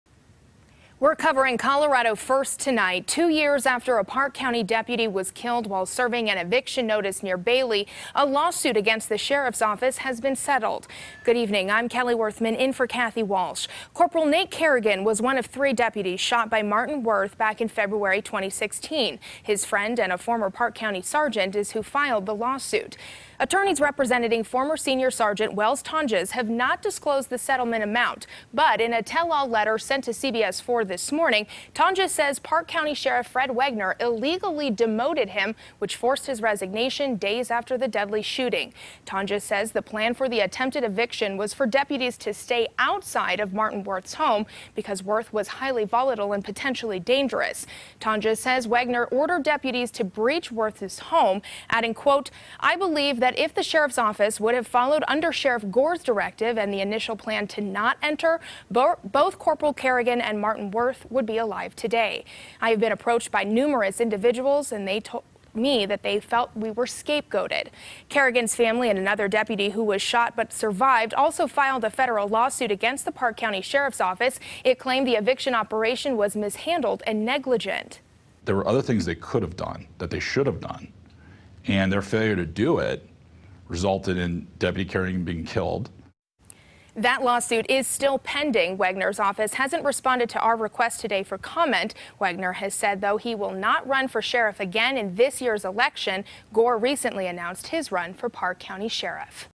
Former Sergeant Releases Tell-All Following Lawsuit Settlement
A former Park County Sheriff's sergeant has settled a lawsuit with the sheriff's office after he was demoted following a shootout that killed Corporal Nate Carrigan.